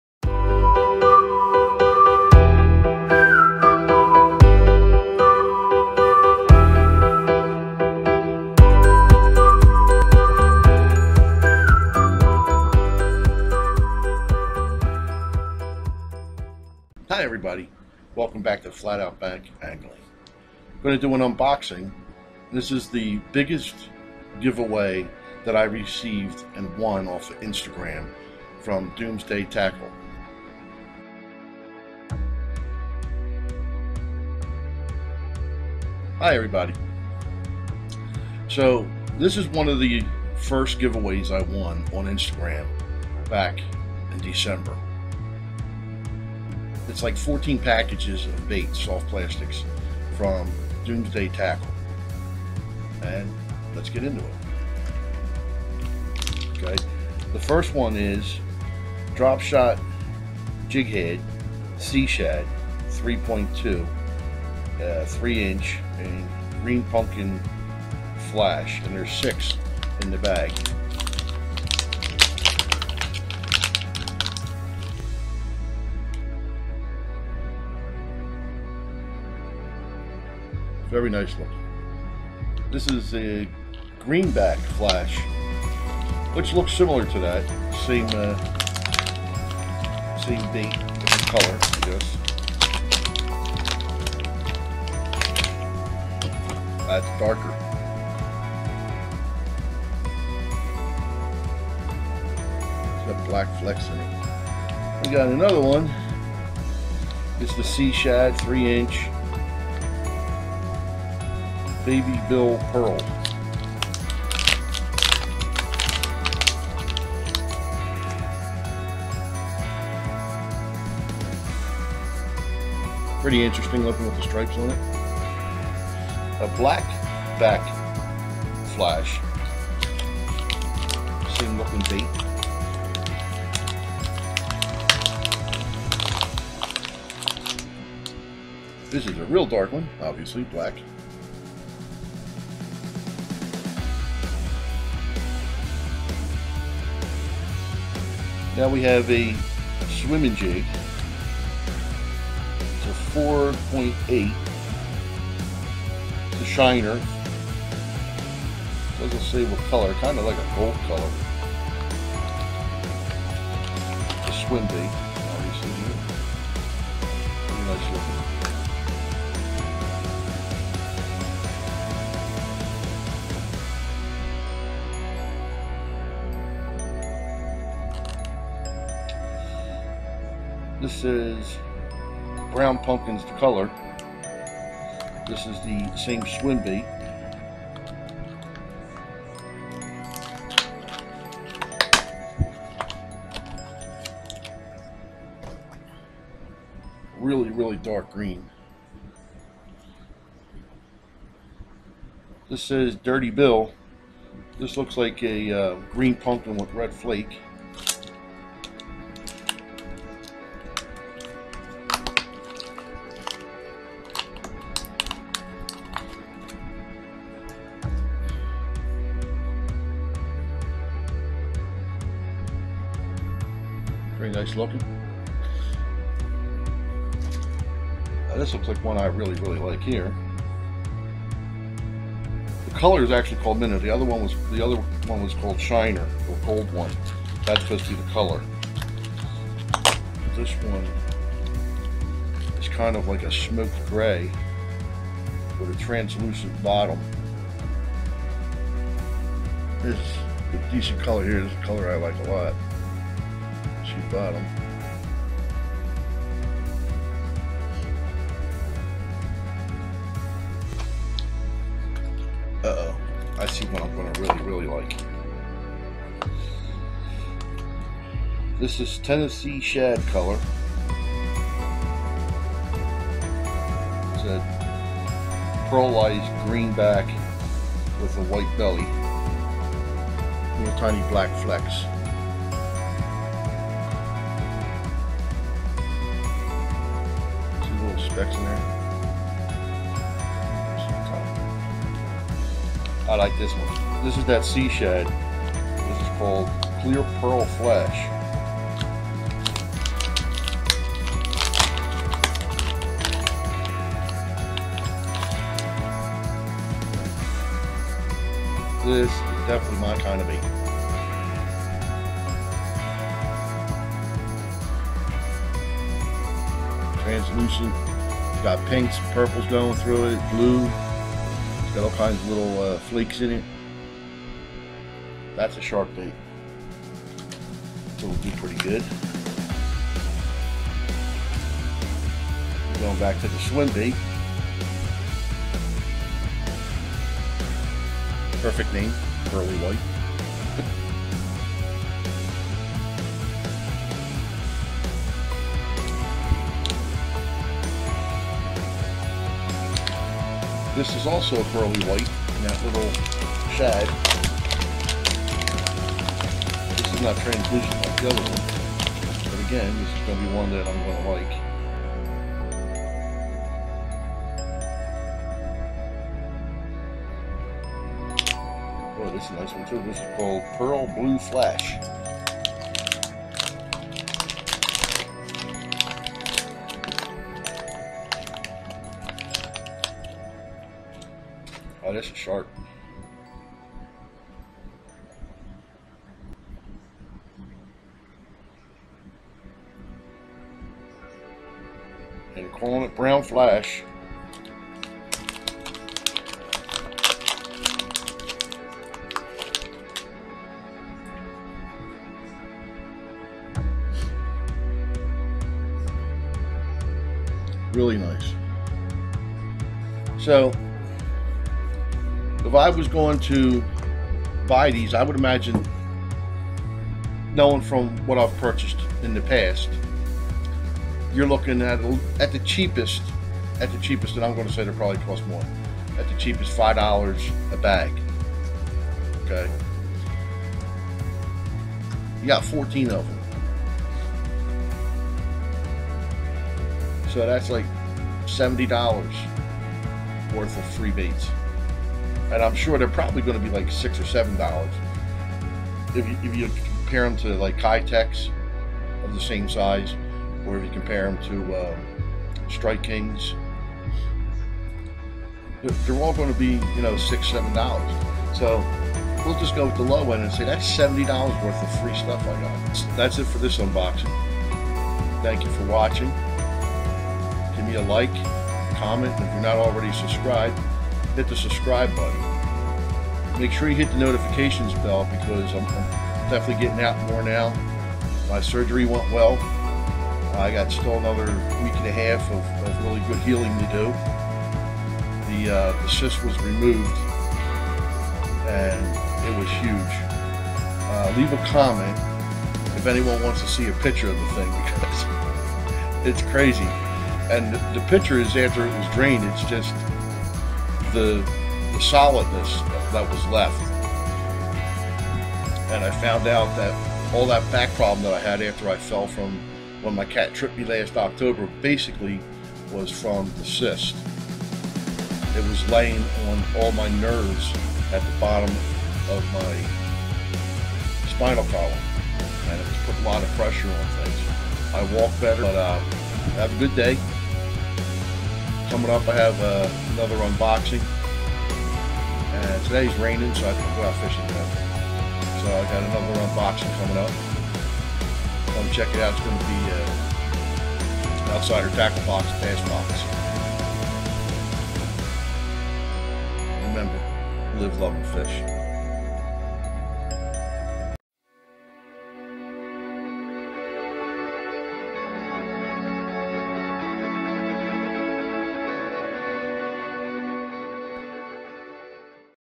DOOMSDAY TACKLE CO. GIVEAWAY WINNINGS
This is a video of  an box opening of baits I won on Instagram in a giveaway by Doomsday Tackle Co.Its a total of 14 packages of soft plastic baits. Check out DoomsdayTackleCo.com to get these baits for yourself.